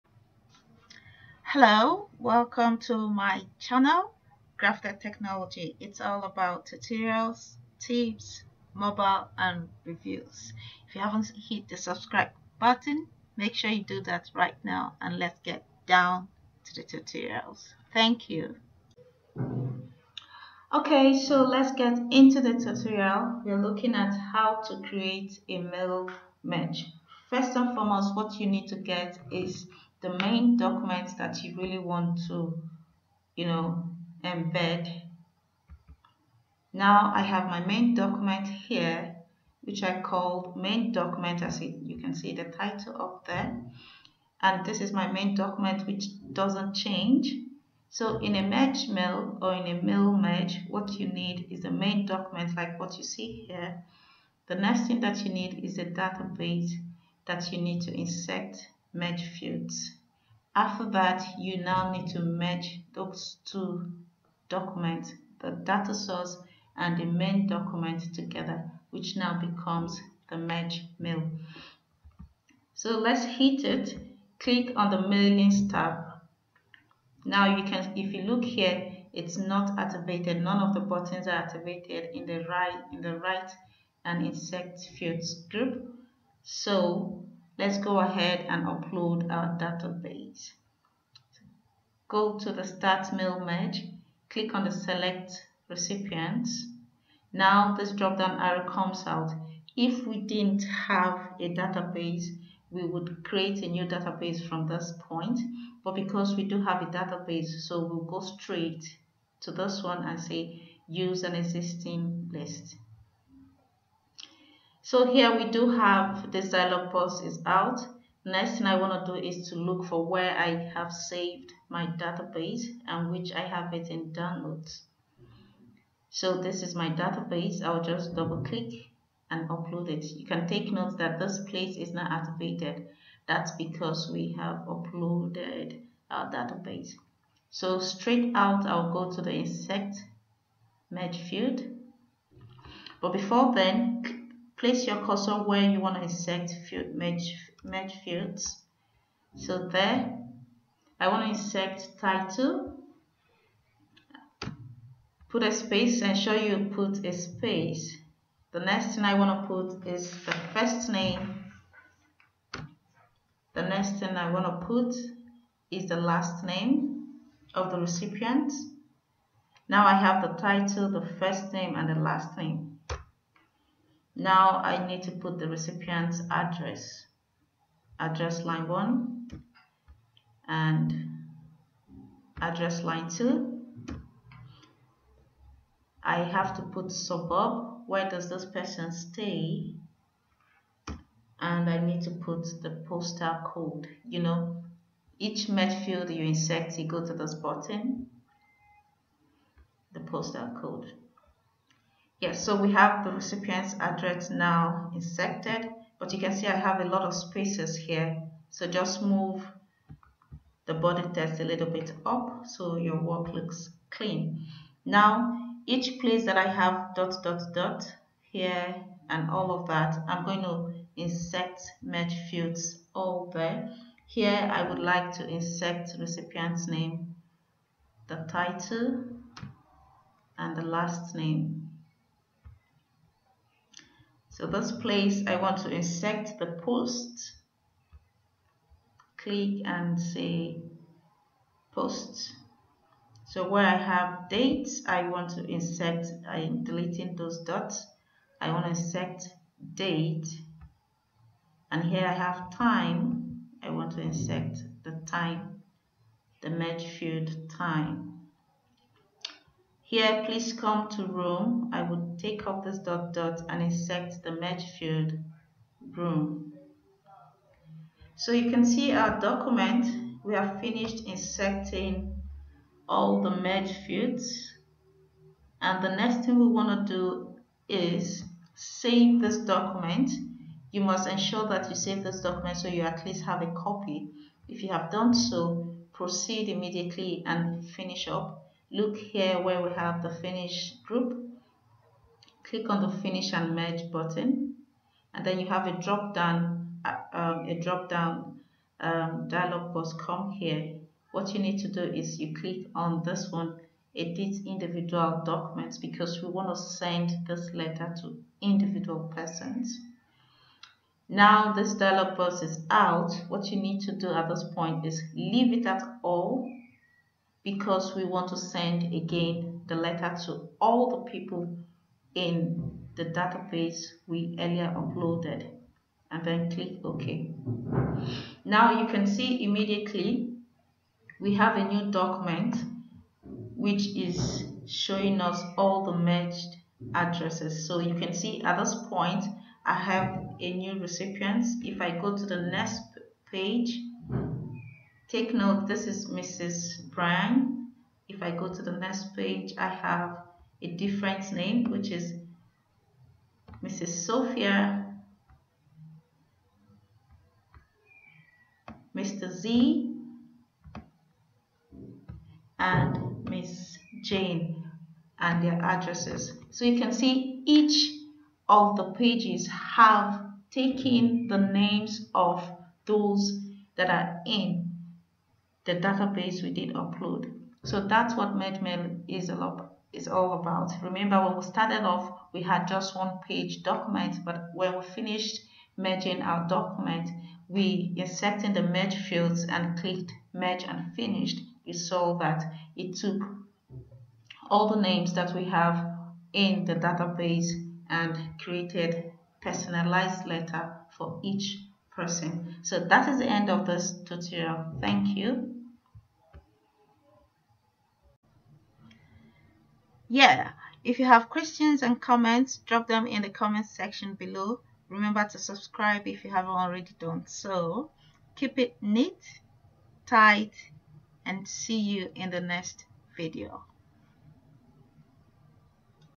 How to Create Mail Merge: Office 365
Mail merge enables you to create some documents, for example, personalized form letter that can be sent to many customers.